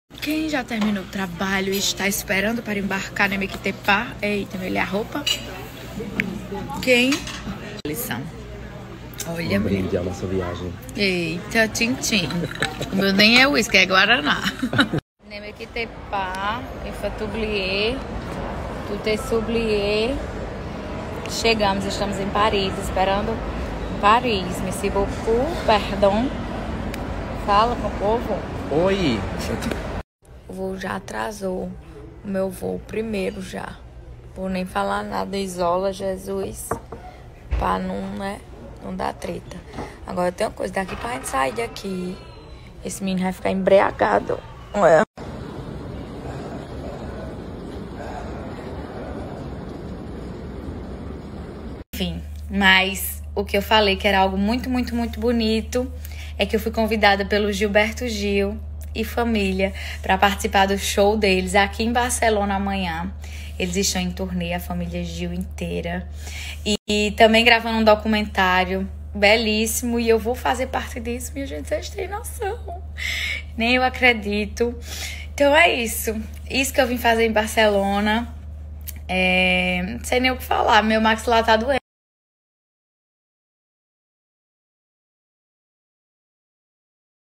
O QUE JULIETTE FOI FAZER NA EUROPA?
Juliette recebe convite inédito para a ir à Europa ela conta tudinho nesse vídeo... Que chique não é mesmo gente?